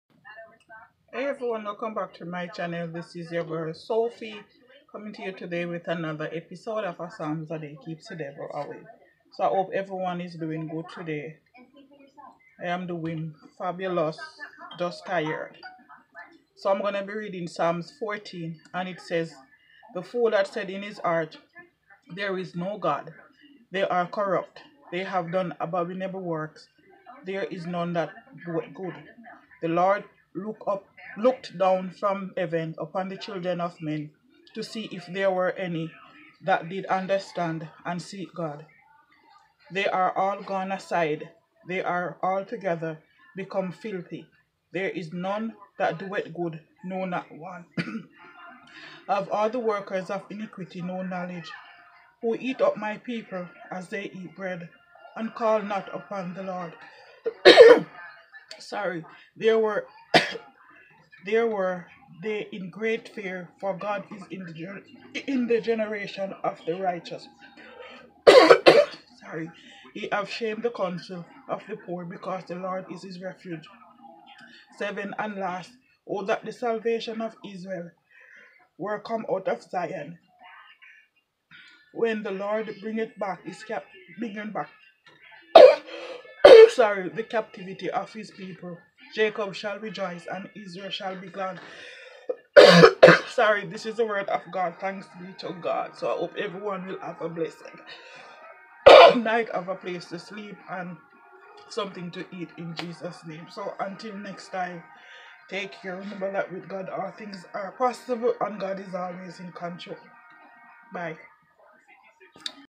A Psalm A Day Keeps The Devil Away
Hey everyone i am back with Psalm a day...
Hope you all will enjoy this one...God bless and keep you all blessed love always.💕💖💗
Am so sorry my throat was giving me alittle trouble..otherwise be bless